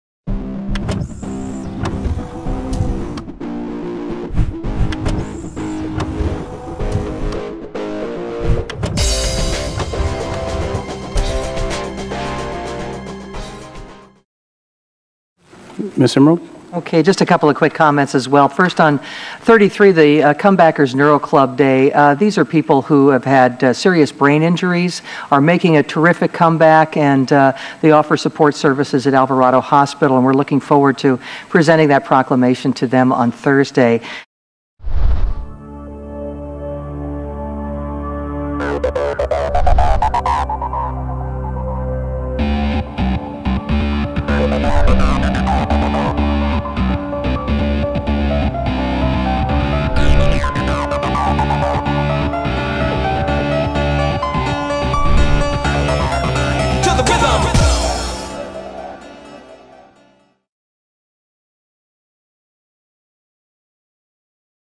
City of San Diego Celebrates Alvarado's Stroke Survivors
The City of San Diego declares Nov. 15, 2011 as Comebackers Neuro Day! This is a celebation of stroke survivors and others who have survived a traumatic brain injury.